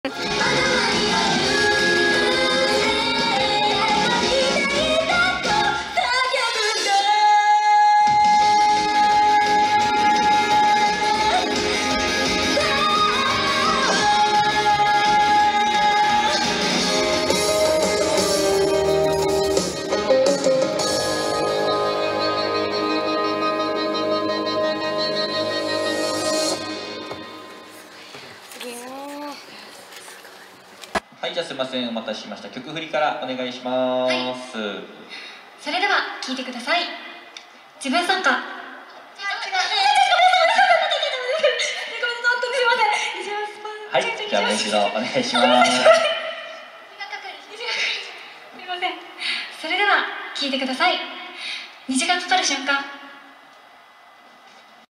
リハから全力歌唱でメンバーをうっとりさせたり、曲振りミスってびっくりさせたり忙しい冨田菜々風 ノイミー
ノイミー
冨田菜々風